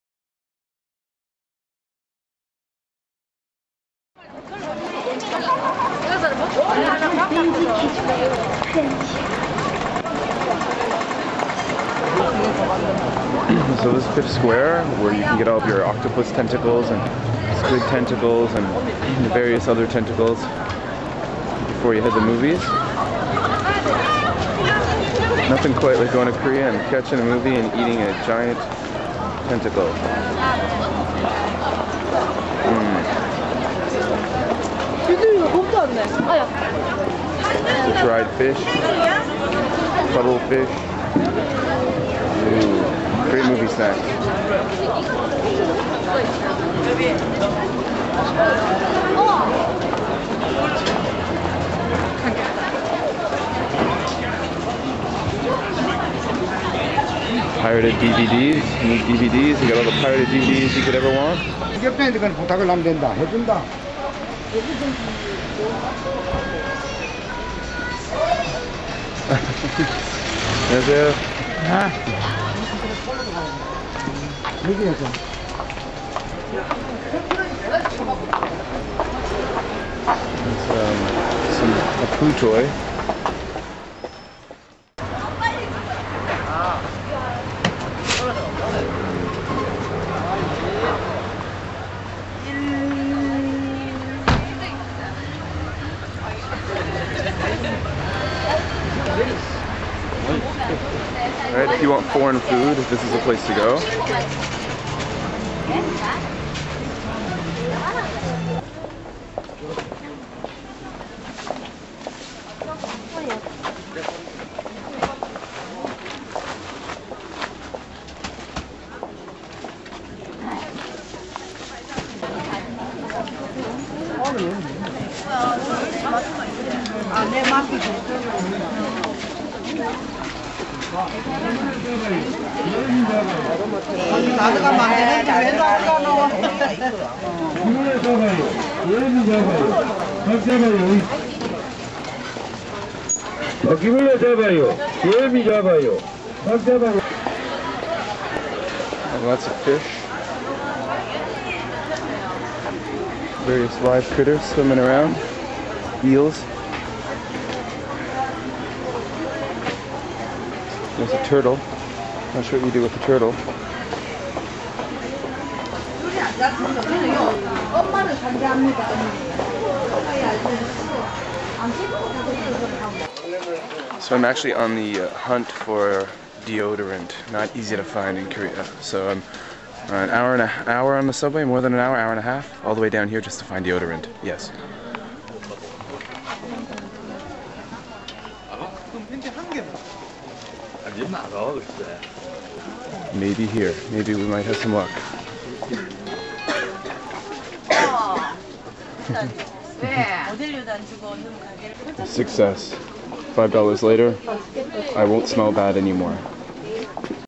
Korean Market in Busan
Earlier in the year I ventured to Nampo-dong market to do some shopping. one of the few places you can buy deoderant in the city. My advice. stock up before you come to Korea!!